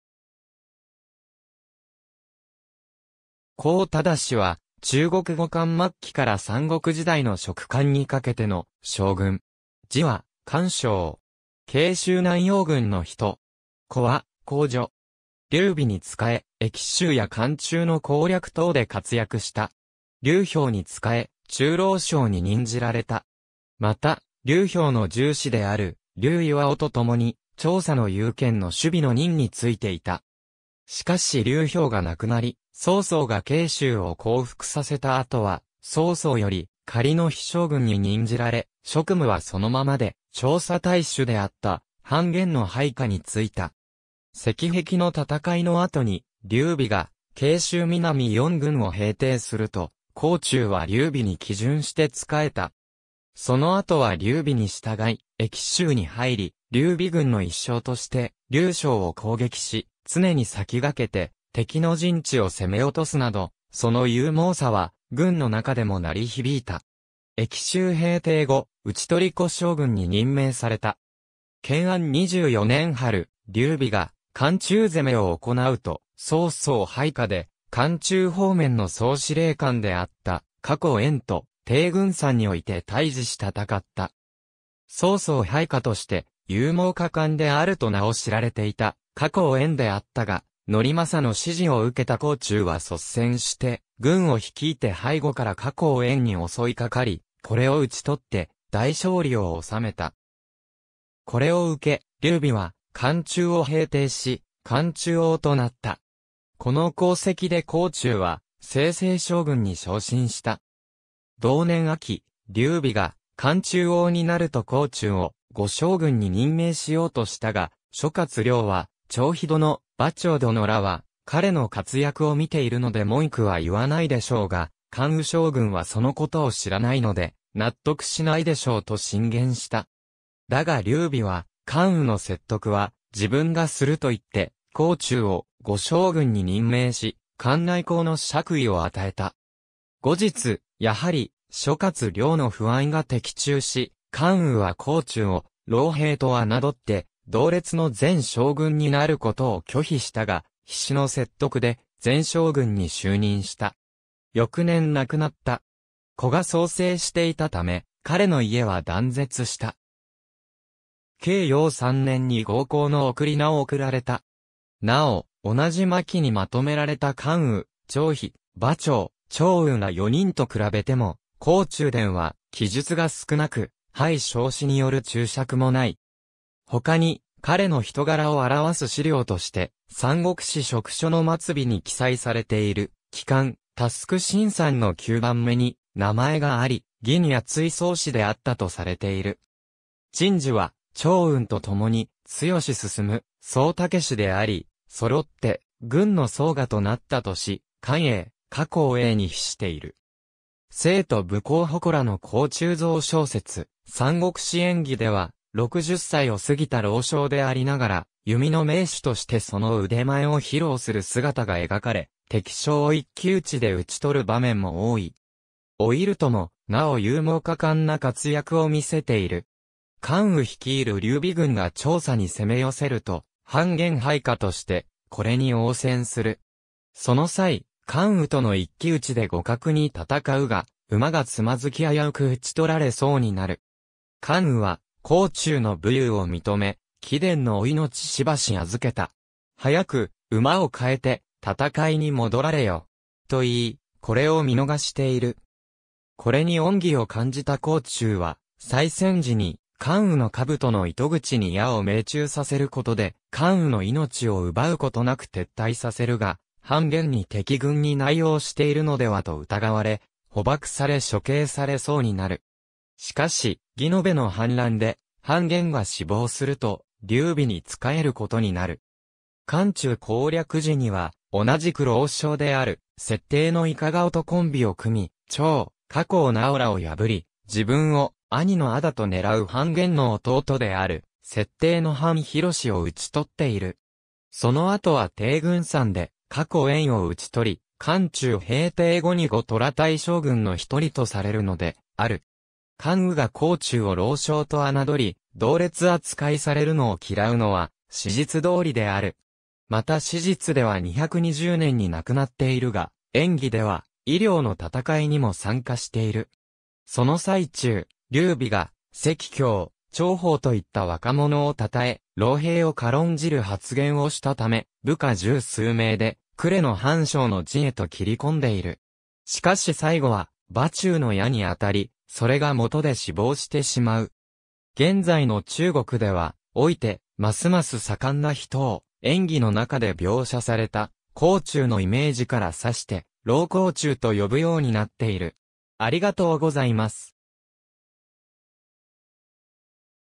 黄忠
黄 忠（こう ちゅう、 拼音: Huáng Zhōng　未詳 - 220年8月）は、中国後漢末期から三国時代の蜀漢にかけての将軍。
字は漢升（かんしょう）。
荊州南陽郡（現在の河南省南陽市）の人。
子は黄敍。
劉備に仕え、益州や漢中の攻略等で活躍した。
劉表に仕え、中郎将に任じられた。
また、劉表の従子である劉磐と共に、長沙の攸県の守備の任についていた。
しかし劉表が亡くなり、曹操が荊州を降伏させた後は、曹操より仮の裨将軍に任じられ、職務はそのままで長沙太守であった韓玄の配下についた。
赤壁の戦いの後に、劉備が荊州南4郡を平定すると、黄忠は劉備に帰順して仕えた。
その後は劉備に従い益州に入り、劉備軍の一将として劉璋を攻撃し、常に先駆けて敵の陣地を攻め落とすなど、その勇猛さは軍の中でも鳴り響いた（劉備の入蜀）。
益州平定後、討虜将軍に任命された。
建安24年（219年）春、劉備が漢中攻めを行なうと、曹操配下で関中方面の総司令官であった夏侯淵と、定軍山において対峙し戦った。
曹操配下として、勇猛果敢であると名を知られていた夏侯淵であったが、法正の指示を受けた黄忠は率先して、軍を率いて背後から夏侯淵に襲いかかり、これを討ち取って大勝利を収めた（定軍山の戦い）。
これを受け、劉備は漢中を平定し漢中王となった。
この功績で黄忠は征西将軍に昇進した。
同年秋、劉備が漢中王になると黄忠を後将軍に任命しようとしたが、諸葛亮は「張飛殿・馬超殿らは彼の活躍を見ているので文句は言わないでしょうが、（荊州に残った）関羽将軍はそのことを知らないので、納得しないでしょう」と進言した。
だが劉備は、関羽の説得は自分がすると言って、黄忠を後将軍に任命し、関内侯の爵位を与えた。
後日、やはり諸葛亮の不安が的中し、関羽は黄忠を「老兵」と侮って同列の前将軍になることを拒否したが、費詩の説得で前将軍に就任した。
翌年（220年）亡くなった。
子が早世していたため、彼の家は断絶した。
景耀3年（260年）に剛侯の諡を送られた。
なお、同じ巻に纏められた関羽・張飛・馬超・趙雲ら四人と比べても黄忠伝は記述が少なく、裴松之による注釈も無い。
他に彼の人柄を表す資料として、『三国志』蜀書の末尾に記載されている『季漢輔臣賛』の9番目に名前があり、「義に厚い壮士」であったとされている。
陳寿は、趙雲と共に彊摯・壮猛であり、揃って軍の爪牙となったとし、灌嬰、夏侯嬰に比している。
成都武侯祠の黄忠像 小説『三国志演義』では、60歳を過ぎた老将でありながら、弓の名手としてその腕前を披露する姿が描かれ、敵将を一騎討ちで討ち取る場面も多い。
老いるともなお勇猛果敢な活躍を見せている。
関羽率いる劉備軍が長沙に攻め寄せると、韓玄配下としてこれに応戦する。
その際、関羽との一騎討ちで互角に戦うが、馬が躓き危うく討ち取られそうになる。
関羽は黄忠の武勇を認め「貴殿のお命しばし預けた。早く馬を換えて戦いに戻られよ。」と言い、これを見逃している。
これに恩義を感じた黄忠は再戦時に、関羽の兜の緒に矢を命中させる事で、関羽の命を奪うことなく撤退させるが、韓玄に敵軍に内応しているのではと疑われ、捕縛され処刑されそうになる。
しかし、魏延の反乱で韓玄が死亡すると、劉備に仕えることになる。
漢中攻略時には、同じく老将である設定の厳顔とコンビを組み、張郃・夏侯尚らを破り、自分を兄の仇と狙う韓玄の弟である設定の韓浩を討ち取っている。
その後は定軍山で夏侯淵を討ち取り、漢中平定後に五虎大将軍の一人とされるのである。
関羽が黄忠を老将と侮り、同列扱いされるのを嫌うのは史実通りである。
また史実では220年に亡くなっているが、『演義』では夷陵の戦いにも参加している。
その最中、劉備が関興・張苞といった若者を称え、老兵を軽んじる発言をしたため、部下十数名で呉の潘璋の陣へと斬り込んでいる。
しかし最後は馬忠の矢にあたり、それが元で死亡してしまう。
現在の中国では、老いて益々盛んな人を、演義の中で描写された黄忠のイメージから指して「老黄忠」と呼ぶ様になっている。